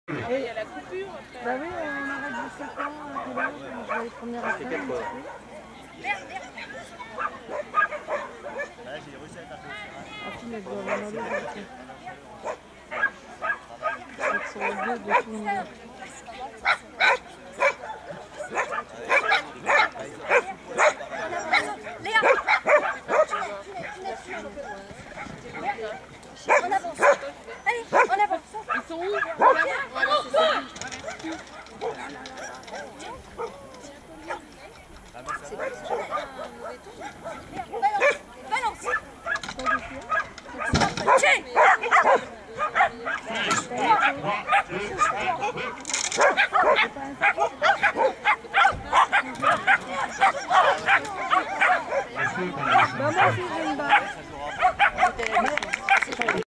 R'LEA Championnat 2011 manche 1 agility 3eme degré cat.C
Championnat de Bourgogne d'agility, Barges 24 avril 2011, 3eme degré manche 1 catégorie C